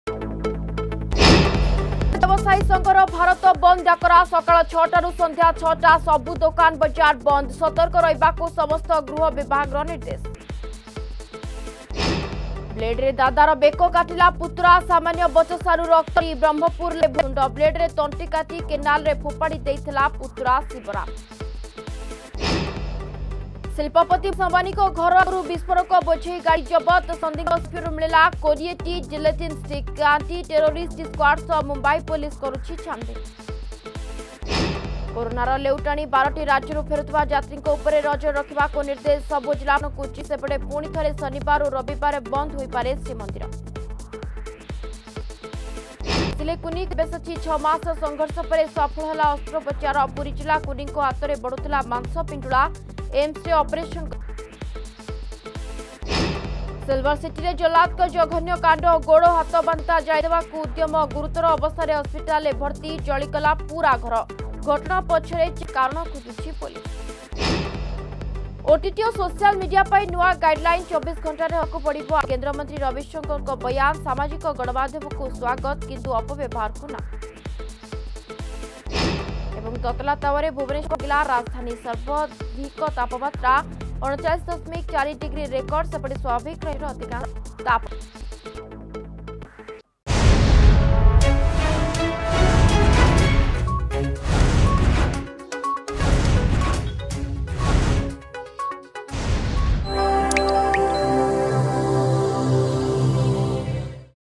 Headlines 9am
ବିଭିନ୍ନ ଦାବିରେ ଆଜି ବ୍ୟବସାୟୀ ସଙ୍ଘର ଭାରତ ବନ୍ଦ ଡାକରା...
ବ୍ଲେଡରେ ଦାଦାର ବେକ କାଟିଲା ପୁତୁରା.
ଶିଳ୍ପପତି ମୁକେଶ ଆମ୍ୱାନିଙ୍କ ଘର ଆଗରୁ ବିସ୍ଫୋରକ ବୋଝେଇ ଗାଡ଼ି ଜବତ.
କୋରୋନାର ଲେଉଟାଣି...
ହସିଲା କୁନି.
ସିଲଭର ସିଟିରେ ଜହ୍ଲାଦଙ୍କ ଜଘନ୍ୟ କାଣ୍ଡ...
ଓଟିଟି ଓ ସୋସିଆଲ ମିଡିଆ ପାଇଁ ନୂଆ ଗାଇଡଲାଇନ...
ତତଲା ତାୱାରେ ଭୁବନେଶ୍ୱର.